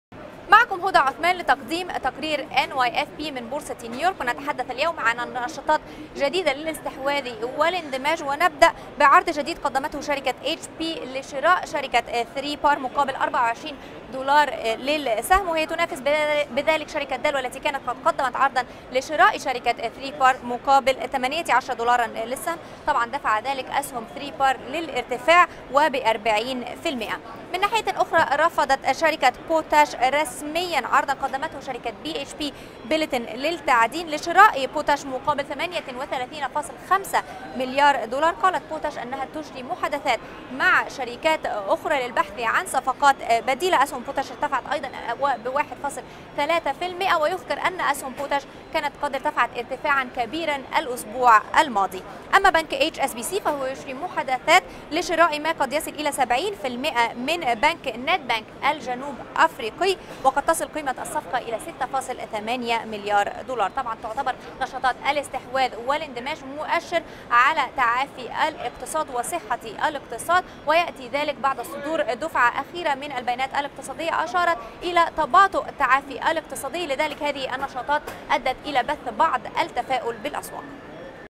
إنتعاش نشاط أم أند آي
08/23/10  Hewlett Packard, Dell, 3Par Inc., HSBC, Nedbank, Potash and BHP Billiton in the headlines.